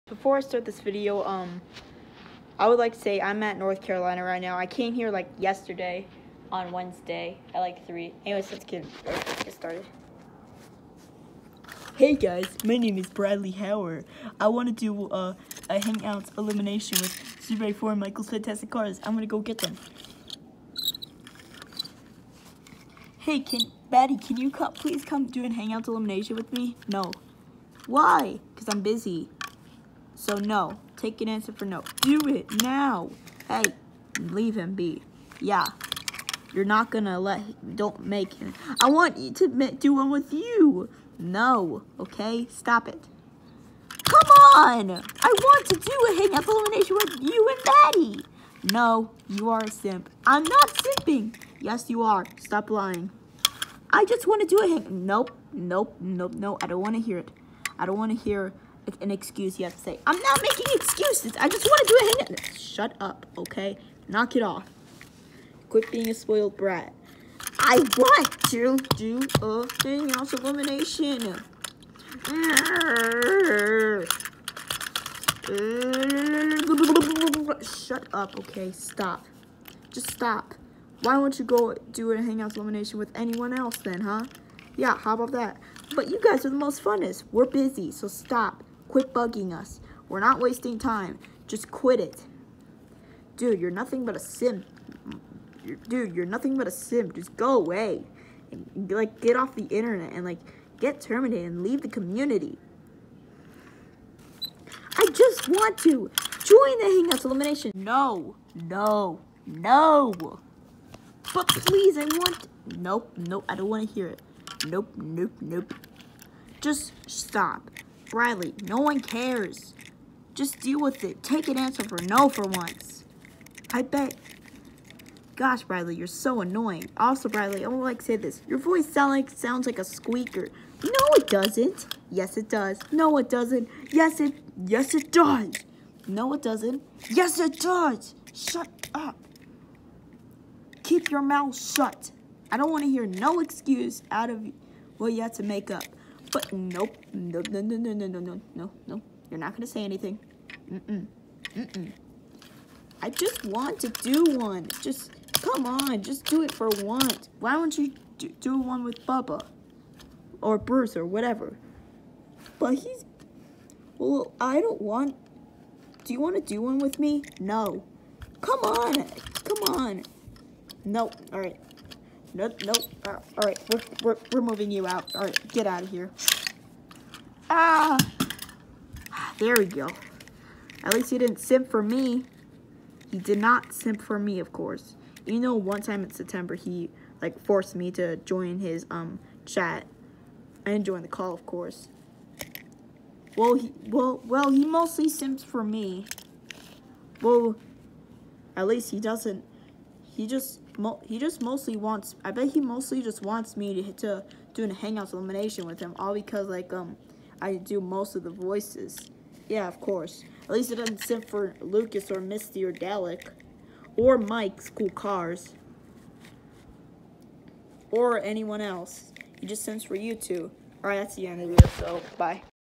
Bradley Hauer In A Nuttshel
He Always Calls A lot On Hangouts The First Time He Started To Call A lot Was Back In July Well Hangouts Got Rid Of Calls So He Cant Call On Hangouts Anymore I Bet He Will Keep Trying To Call On Google Meet And Everyone Time We Add He Always Gives Us A Link To His Hangouts Chat Witch Is So Annoying